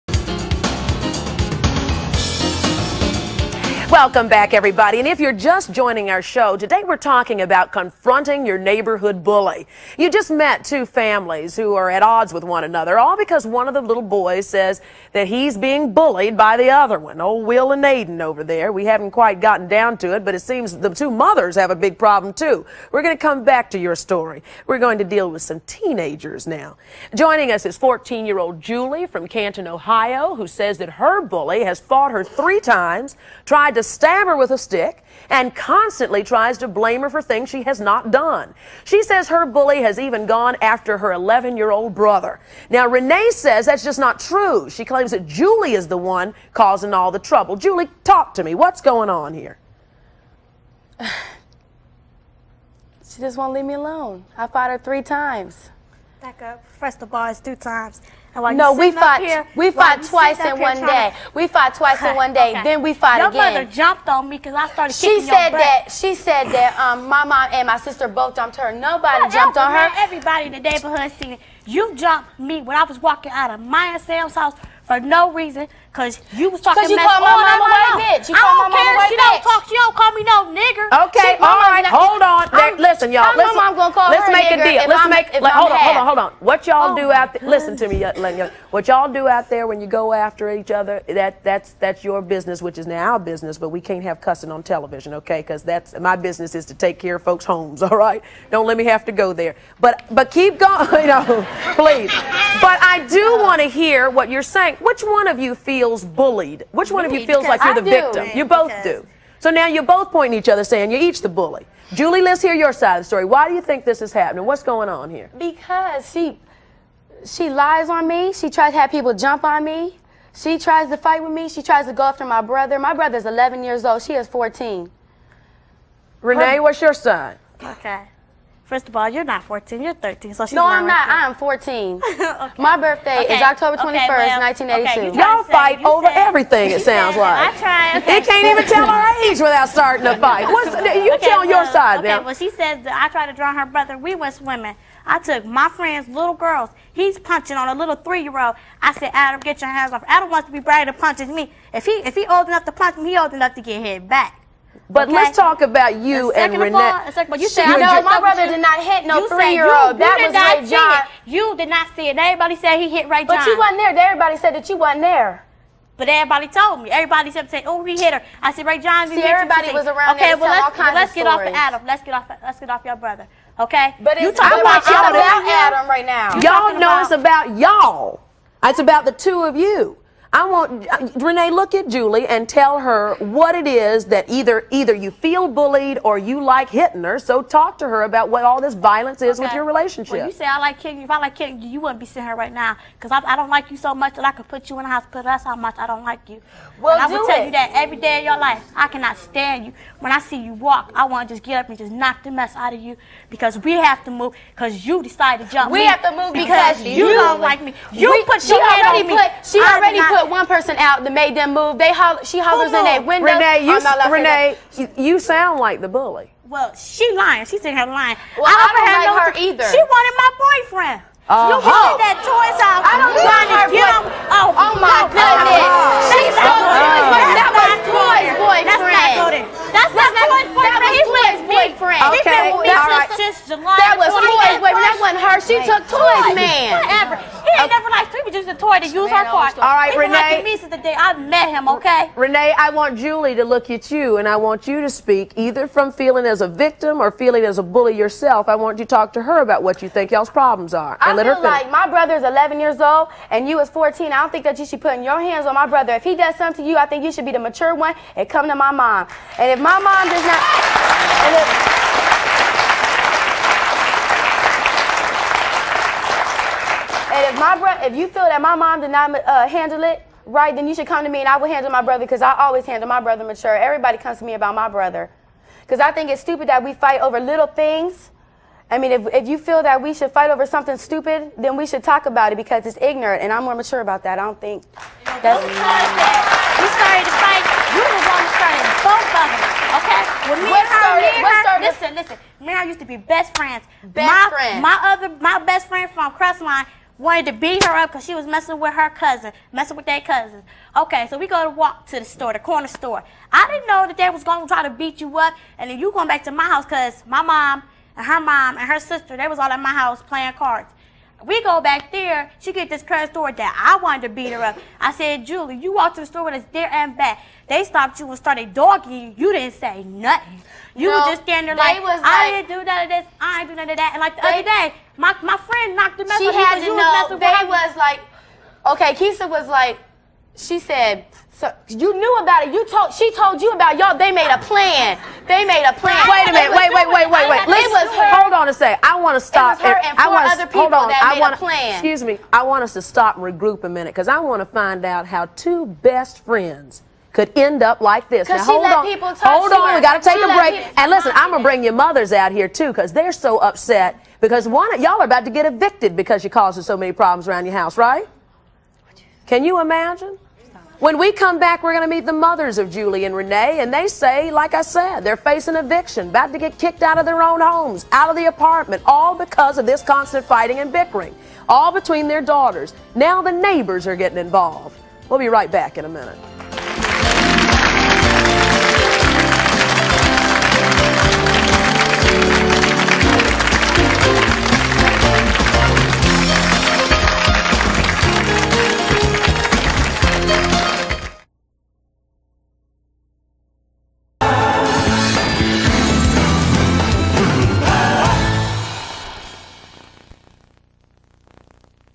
Confronting Your Neighborhood Bully (Part 2)
Ways for kids to confront their neighborhood bullies, while at school. Who to go to, and what to do if they are being bullied!!